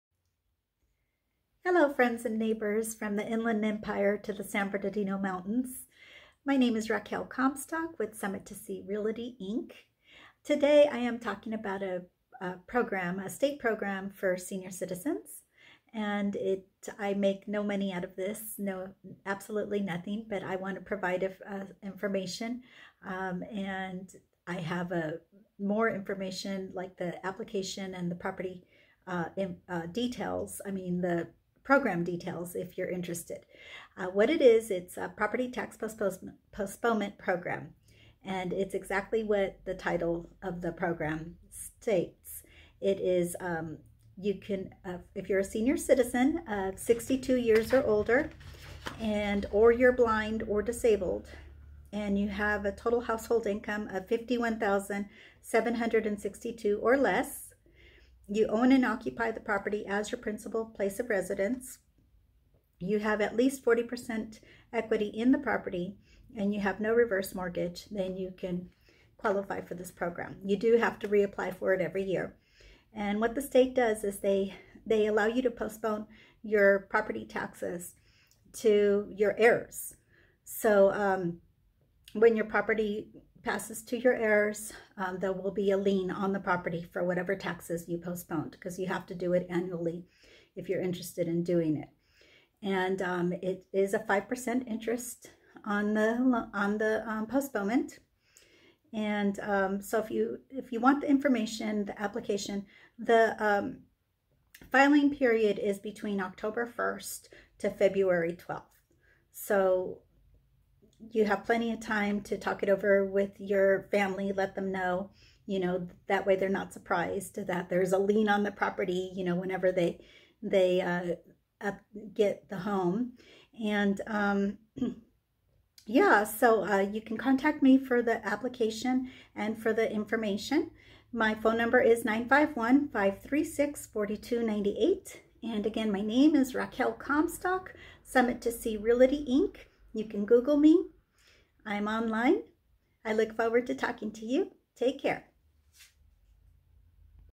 California Property Tax Postponement Program
Get free info about the CA PropertyTax Postponement program for seniors who are at least 62, blind or disabled.  The income limit is 51,762.  No reverse mtg allowed.  Primary residence required.  you must have at least 40% equity.  The filing period is between October 1st to Feb. 12th.

The state charges you 5% interest added as a lien on your property and it is passed onto your heirs.  I am not a representative of this program and I receive no benefits from sharing this information.

Moving mountains to sell you a home since 1990, from the Inland Empire, CA to the San Bernardino Mountains.